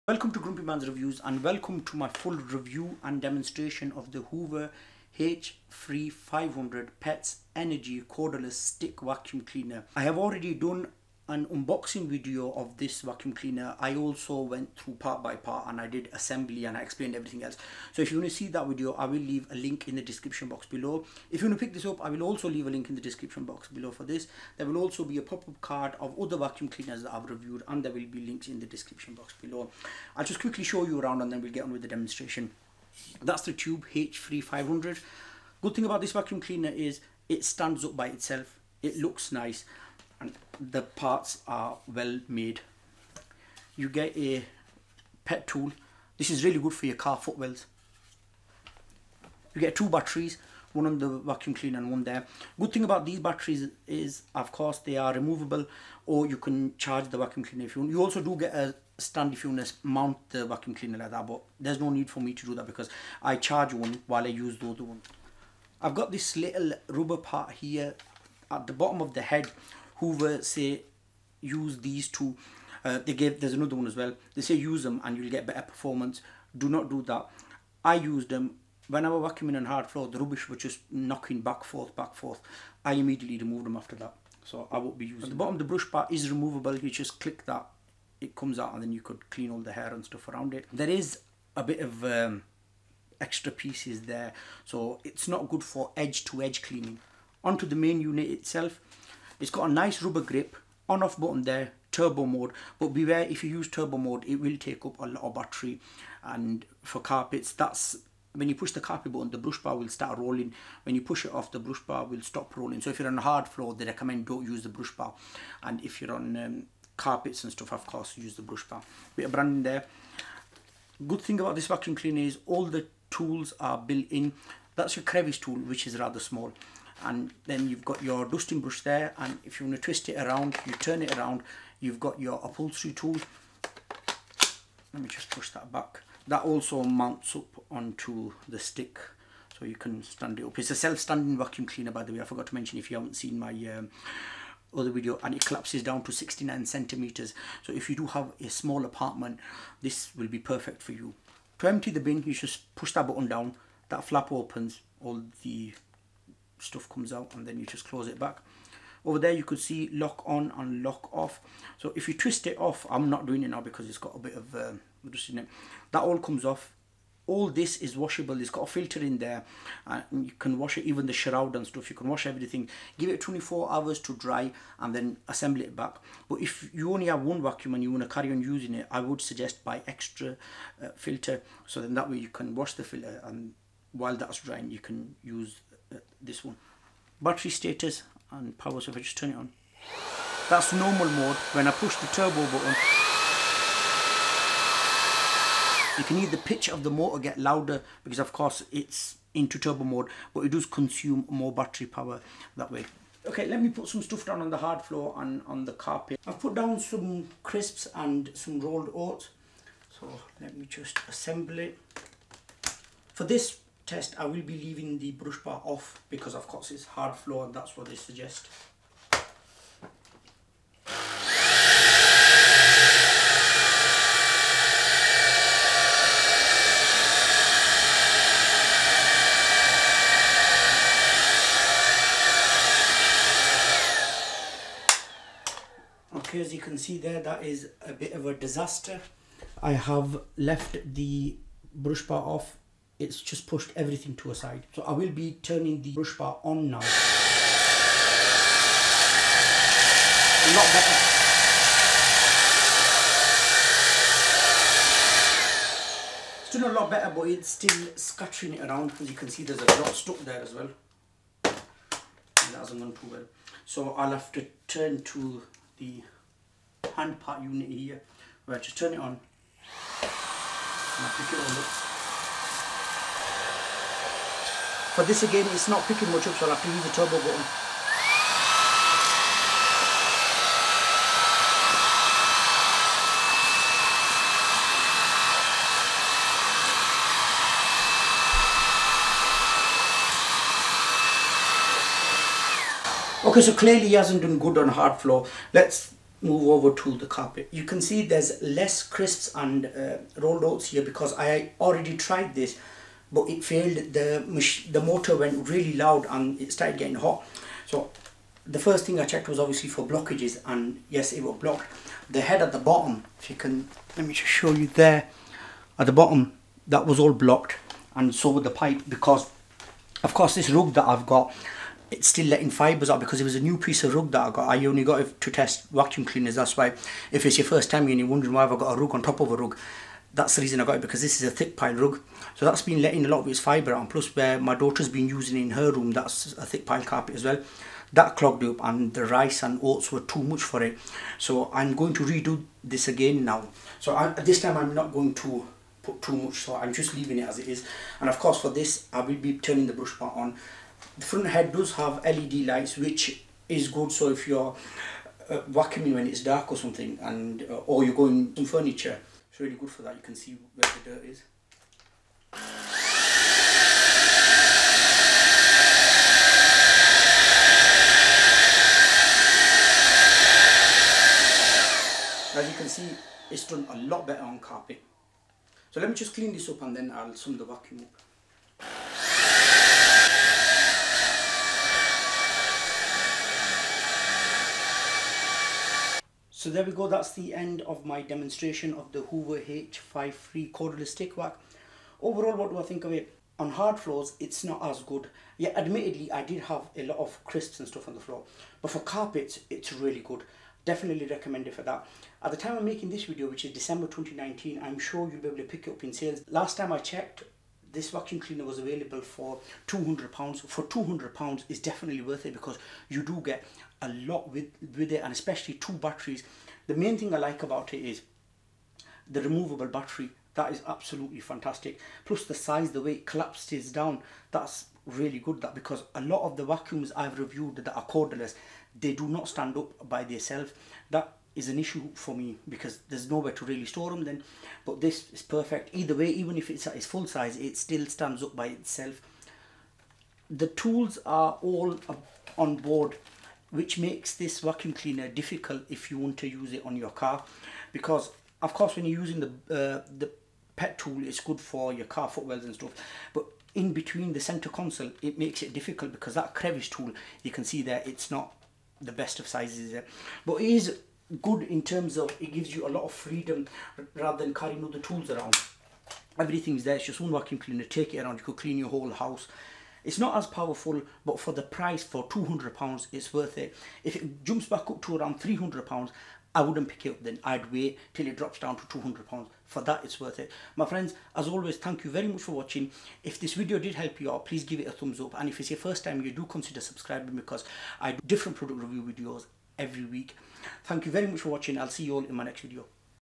Hoover H Free 500 Cordless Vacuum Cleaner Review & Demonstration 2020
Demonstration and review of the Hoover H-FREE 500 Pets Energy 3in1 Cordless Stick Vacuum Cleaner, HF522PTE.

Get your Hoover H-FREE 500 Pets Energy Cordless Stick Vacuum Cleaner